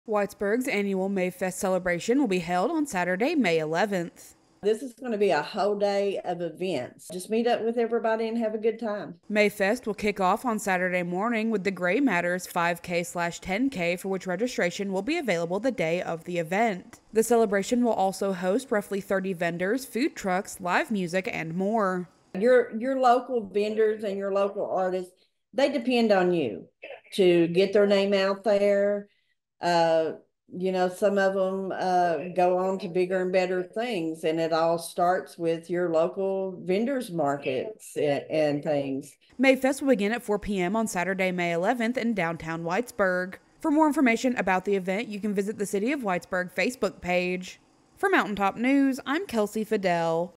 Mayfest Returns to Whitesburg on May 11, 2024
Mayfest will be returning to Whitesburg, Kentucky, on Saturday, May 11, 2024.